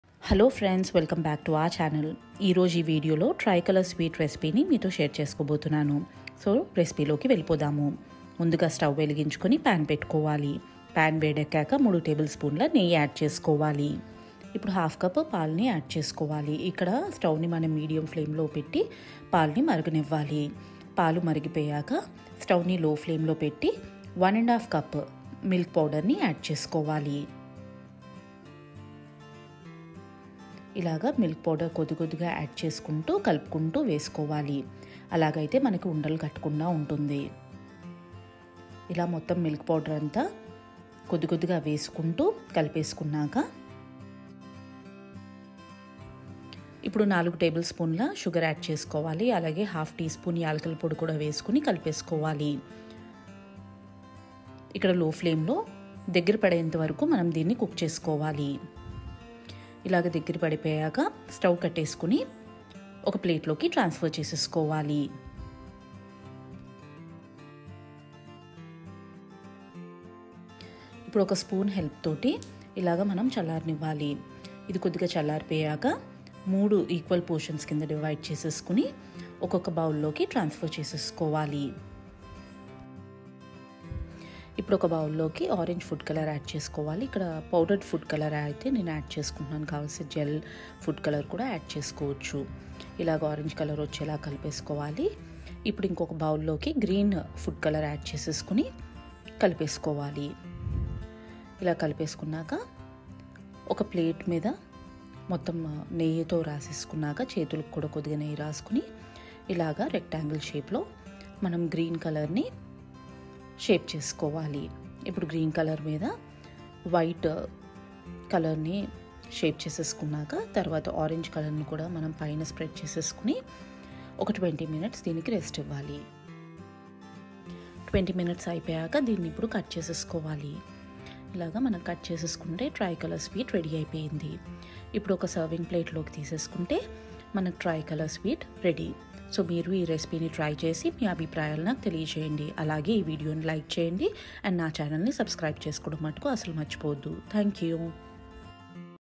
Tri colour sweet recipe | Republic day special sweet | haristastic kitchen
Tri colour sweet recipe

This tri colour sweet is prepared by milk powder it is easy to prepare and tastes good as well

Do try this recipe recipe

And give ur comments

For more recipes do checkout my YouTube channel haristastic kitchen